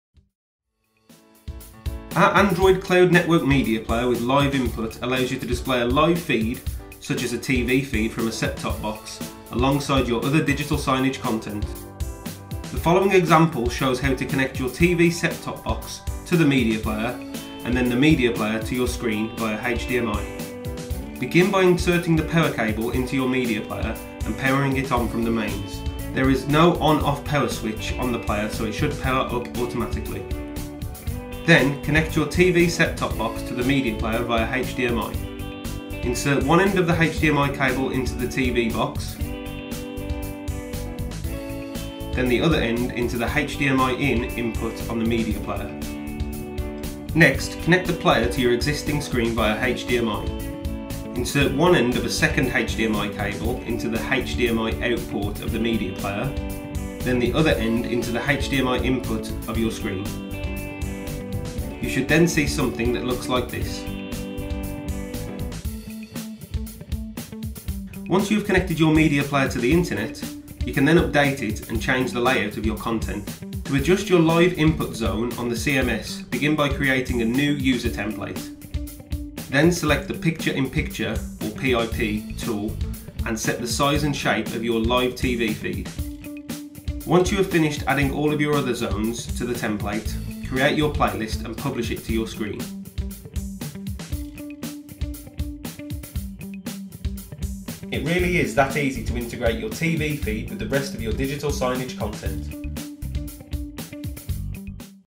Displaying Live TV with Digital Signage
Connecting the Cloud Network Media Player to your screen via HDMI or VGA allows you to update your screen either wirelessly (via a Wi-Fi network) or via a Local Area Network (LAN) connection; both options are available with this media player. We can also offer a 4G solution if the location does not already have internet access.Their integrated Android PC board provides a super fast processing speed allowing for improved communication with the CMS.
This means that you can update your screens quicker than ever before. It also allows you to display a live website; this can be anything from a live URL feed provided by a third party to your social media profile.